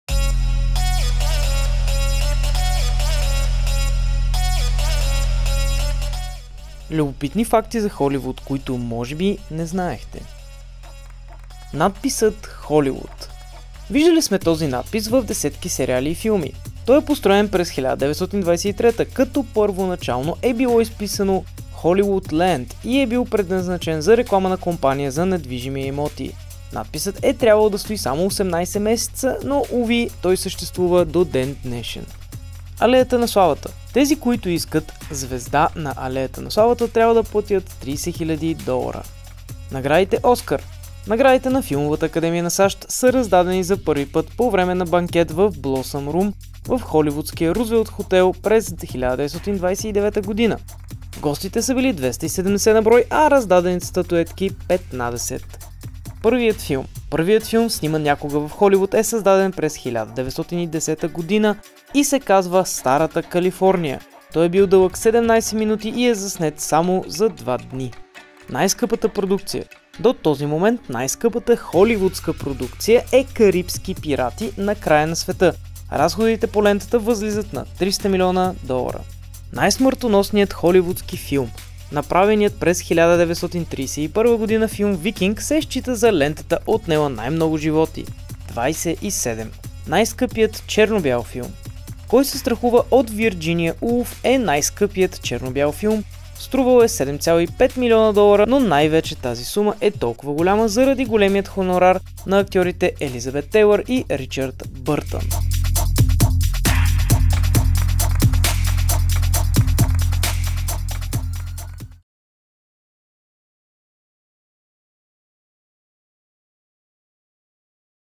Любопитни факти за Холивуд, които може би не знаехте
Надписът „Hollywood“

Виждали сме този надпис в десетки сериали и филми. Той е построен построен през 1923, като първоначално е било изписано Hollywoodland  и е бил предназначен за реклама на компания за недвижими имоти. Надписът е трябвало да стои само 18 месеца, но, уви, той съществува до ден днешен.

Алеята на славата

Тези, които искат звезда на Алеята на славата, трябва да платят $30 000.

Наградите „Оскар“

Наградите на филмовата академия на САЩ са раздадени за първи път по време на банкет в Блосъм рум в холивудския Рузвелт хотел през 1929 година. Гостите са били 270 на брой, а раздадените статуетки 15.

Първият филм

Първият филм, сниман някога в Холивуд е създаден през 1910 година и се казва "Старата Калифорния" . Той е бил дълъг 17мин. и е заснет само за два дни.

Най-скъпата продукция

До този момент най-скъпата холивудска продукция е "Карибски пирати: На края на света" . Разходите по лентата възлизат на $300млн.

Най-смъртоносният холивудски филм

Направеният през 1931 година филм „Викинг“ се счита за лентата, отнела най-много животи: 27.

Най-скъпият черно-бял филм

„Кой се страхува от Вирджиния Улф“ е най-скъпият черно-бял филм. Струвал е 7.5 млн. Долара, но най-вече тази сума е толкова голяма, заради големият хонорар на актьорите Елизабет Тейлър и Ричард Бъртан,